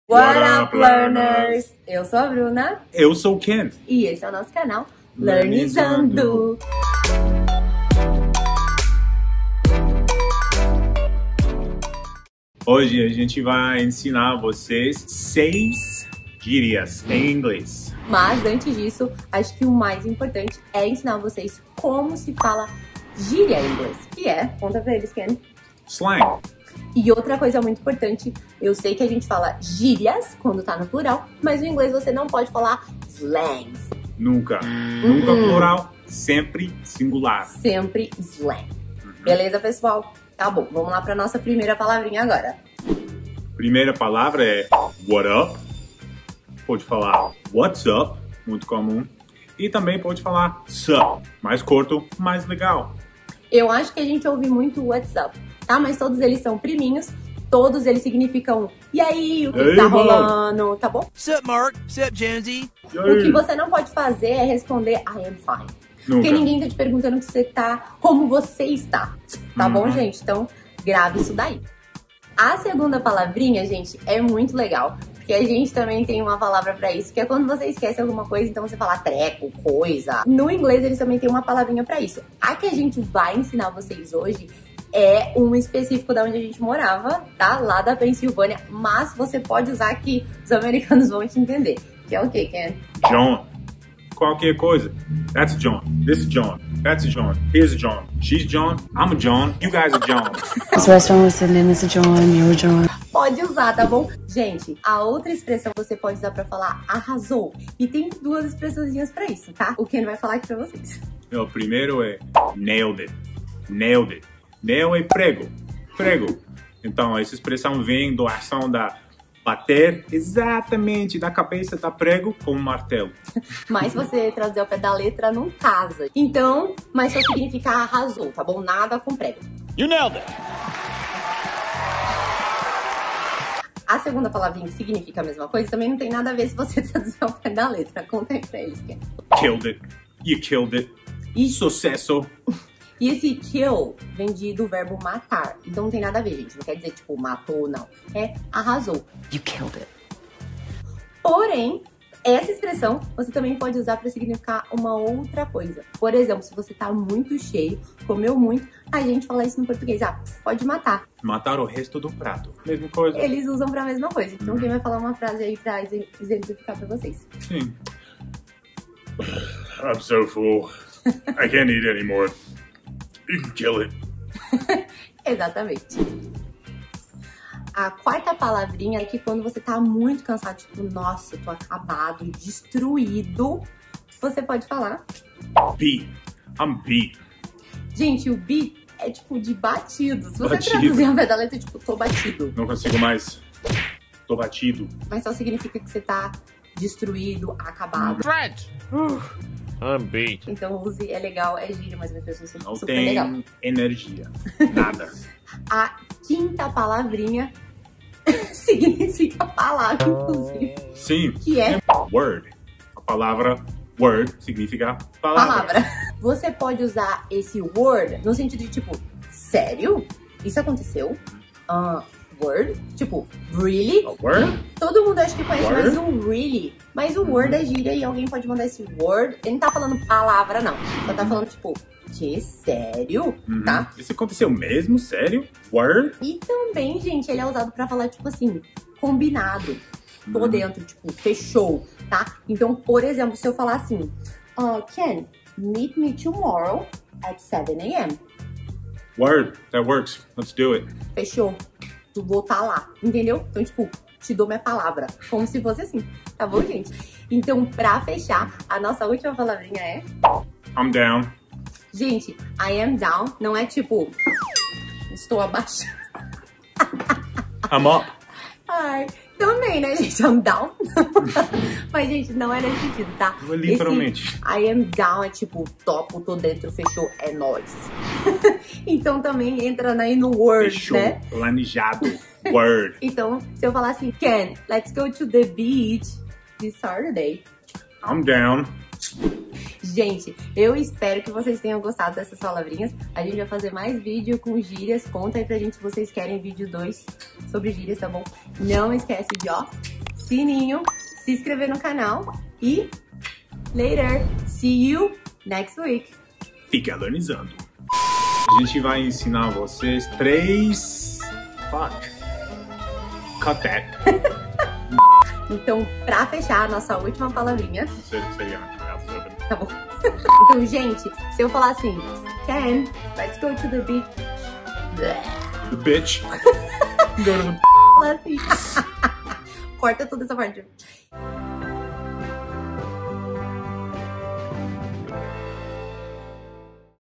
GÍRIAS em INGLÊS | seja mais DESCOLADO
Whadap, learners!!

No vídeo de hoje, vamos ensinar a vocês algumas gírias que os americanos usam no dia a dia. Se você quiser mais conteúdos desse tipo, comenta ai em baixo "quero parte 2" ✌🏼

Não se esqueça de avaliar o video! 👍🏼
See youuu! 😉

# kenseminsta